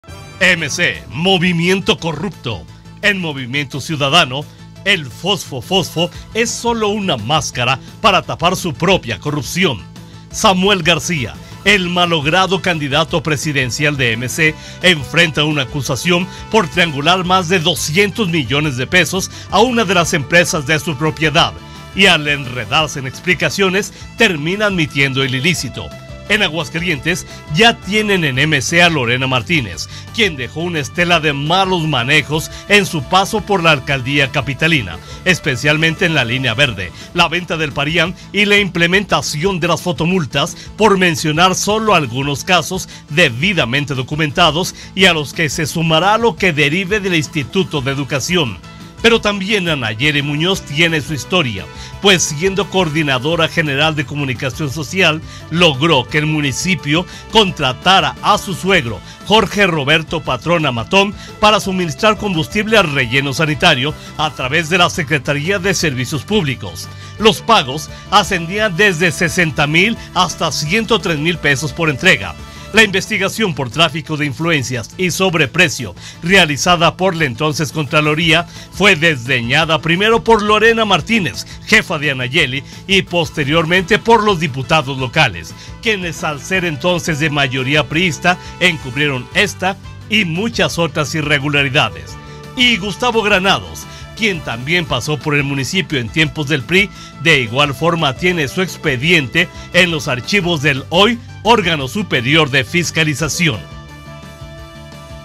Crónica del poder/MC. Movimiento Corrupto
MC. Movimiento Corrupto

Aguascalientes, Ags. 15 marzo 2024.- En Movimiento Ciudadano lo fosco fofo es solo una máscara para tapar su propia corrupción.
Samuel García, el malogrado candidato presidencial de MC, enfrenta una acusación por triangular más de 200 millones de pesos a una de sus empresas. Y al enredarse en explicaciones, termina admitiendo el ilícito.
En Aguascalientes ya tienen en MC a Lorena Martínez, quien dejó una estela de malos manejos en su paso por la alcaldía capitalina, especialmente en la Línea Verde, la venta del Parián y la implementación de las fotomultas, por mencionar solo algunos casos debidamente documentados y a los que se sumará lo que derive del Instituto de Educación.
Pero también Anayeli Muñoz tiene su historia, pues siendo Coordinadora General de Comunicación Social logró que el Municipio contratara a su suegro Jorge Roberto Patrón Amatón para suministrar combustible al relleno sanitario, a través de la Secretaría de Servicios Públicos.
Los pagos, ascendían desde los 60 mil hasta los 103 mil pesos por entrega.
La investigación por tráfico de influencias y sobreprecio, realizada por la entonces Contraloría, fue desdeñada primero por Lorena Martínez, jefa de Anayeli, y posteriormente por los diputados locales, quienes al ser entonces de mayoría priísta encubrieron ésta y muchas otras irregularidades.
Y Gustavo Granados, quien también pasó por el Municipio en tiempos del PRI de igual forma tiene su expediente en los archivos del hoy Órgano Superior de Fiscalización.